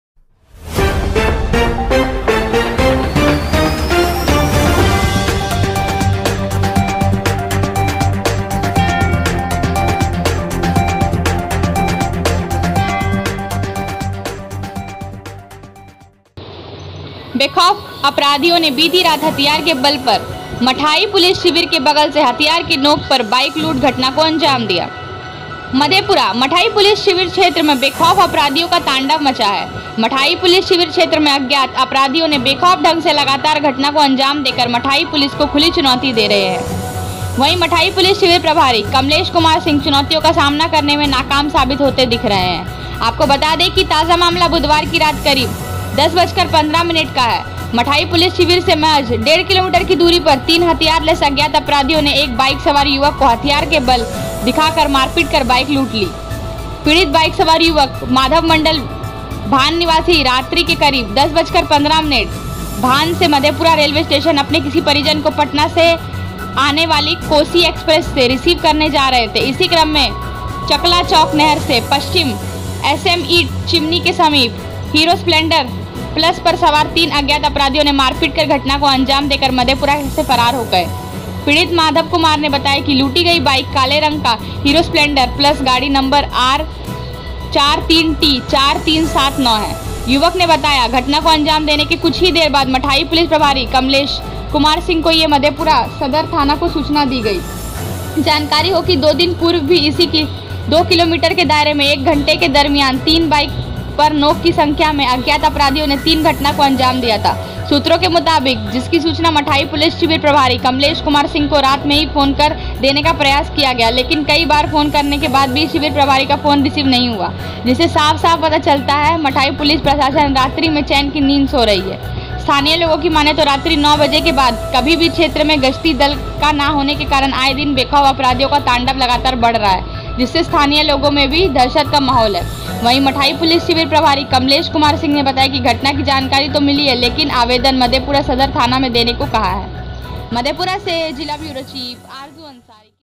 बेखौफ अपराधियों ने बीती रात हथियार के बल पर मट्ठाही पुलिस शिविर के बगल से हथिया...|jankari junction
बेखौफ अपराधियों ने बीती रात हथियार के बल पर मट्ठाही पुलिस शिविर के बगल से हथियार के नोक पर बाइक लूट घटना को दिया अंजाम

मधेपुरा: मठाही पुलिस शिविर क्षेत्र में बेखौफ अपराधियों का तांडव मची है मठाही पुलिस शिविर क्षेत्र में अज्ञात अपराधियों ने बेखौफ ढंग से लगातर घटना को अंजाम देकर मठाही पुलिस को खुली चुनौती दे रही है । वही मठाही पुलिस शिविर प्रभारी कमलेश कुमार सिंह चुनौतियों का सामना करने में नाकाम साबित होते दिख रहे हैं। आपको बता दें कि ताजा मामला बुधवार की रात करीब 10:15 बजे मठाही पुलिस शिविर से महज डेढ़ किलोमीटर की दूरी पर तीन हथियार लेस अज्ञात अपराधियों ने एक बाइक सवार युवक को हथियार का बल दिखा मारपीट कर बाइक लूट ली। पीड़ित बाइक सवार युवक माधव मंडल भान निवासी रात्रि के करीब 10:15 बजे भान से मधेपुरा रेलवे स्टेशन अपने किसी परिजन को पटना से आने वाली कोसी एक्सप्रेस से रिसीव करने जा रहे थे इसी क्रम में चकला चौक नहर से पश्चिम एस एम ईट चिमनी के समीप हीरो स्प्लेंडर प्लस पर सवार तीन अज्ञात अपराधियों ने मारपीट कर घटना को अंजाम दे कर मधेपुरा की फरार हो गया। पीड़ित माधव कुमार ने बताया की लुटी गई बाइक काले रंग का हीरो स्प्लेन्डर प्लस गाड़ी नम्बर बी आर 43 टी 4379 है । युवक ने बताया घटना को अंजाम देने के कुछ ही देर बाद मठाही पुलिस प्रभारी कमलेश कुमार सिंह को एवं मधेपुरा सदर थाना को सूचना दे दी गई। जानकारी हो कि 2 दिन पूर्व भी इसी 2 किलोमीटर के दायरे में 1 घंटे के दरमियान तीन बाइक पर नो की संख्या में अज्ञात अपराधियों ने तीन घटना को अंजाम  दिया था। सूत्रों के मुताबिक जिसकी सूचना मठाही पुलिस शिविर प्रभारी कमलेश कुमार सिंह को रात में ही फोन कर देने का प्रयास किया गया लेकिन कई बार फोन करने बाद भी शिविर प्रभारी का फोन रिसीव नहीं हुआ । जिससे साफ साफ पता चलता है मठाही पुलिस प्रशासन रात्रि में चैन की नींद सो रहे थे।  स्थानीय लोगों की मानें तो रात्रि 9:00 बजे के बाद कभी भी क्षेत्र में गश्ती दल ना होने के कारण आए दिन बेखौफ अपराधियों का तांडव लगातार बढ़ रहा है जिससे स्थानीय लोगों में भी दहशत का माहौल है। वहीं मठाही पुलिस शिविर प्रभारी कमलेश कुमार सिंह ने बताया कि घटना की जानकारी तो मिली है लेकिन आवेदन मधेपुरा सदर थाना में देने को कहा गया ।

मधेपुरा से जिला ब्यूरो चीफ आरजू अंसारी की रिपोर्ट